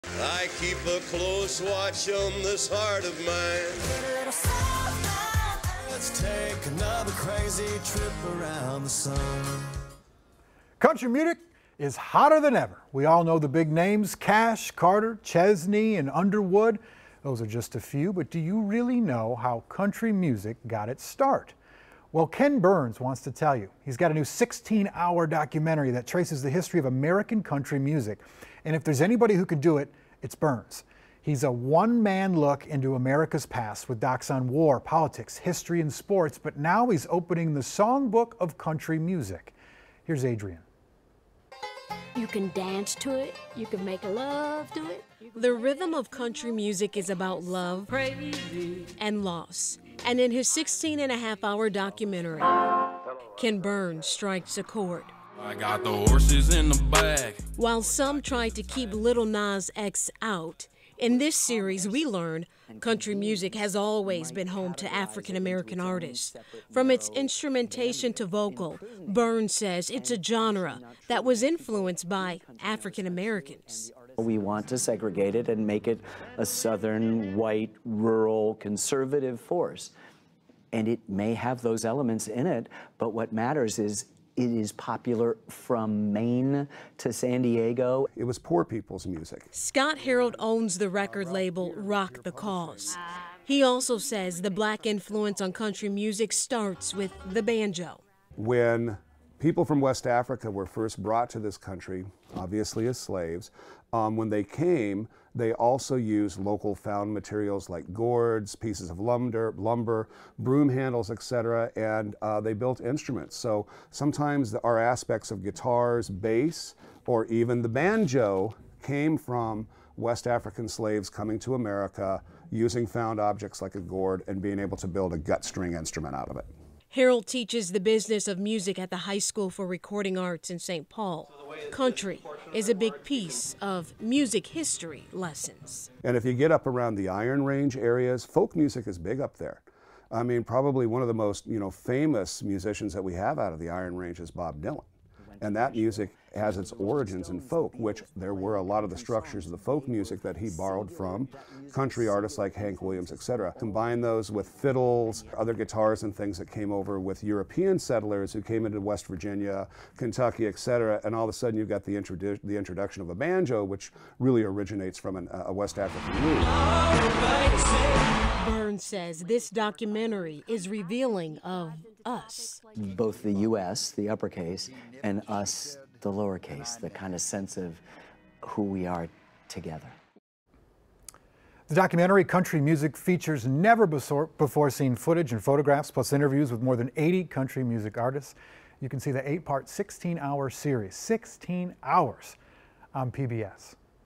New Ken Burns film sheds light on history of country music
'Country Music' features never-before-seen footage and photographs, plus interviews with more than 80 country music artists. The documentary will air on PBS.